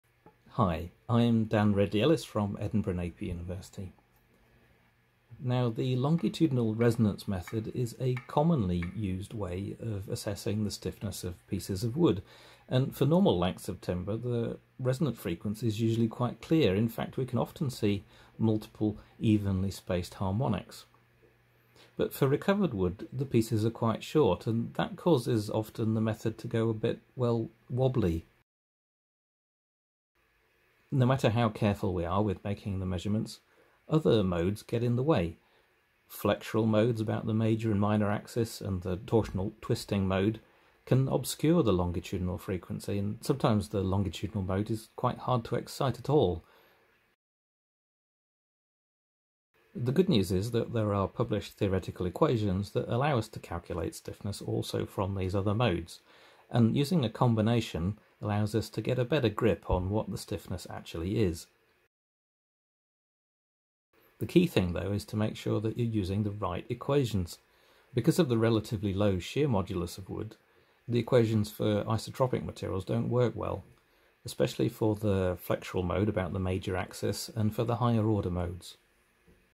WSE2023 video abstract: "Notes on the resonance method for secondary timber"
Ridley-Ellis D “Notes on the resonance method for secondary timber”

A video abstract for  the19th Annual Meeting of the Northern European Network for Wood Science and Engineering (WSE 2023), held in Ås and Oslo, Norway, from 10th to 12th October 2023.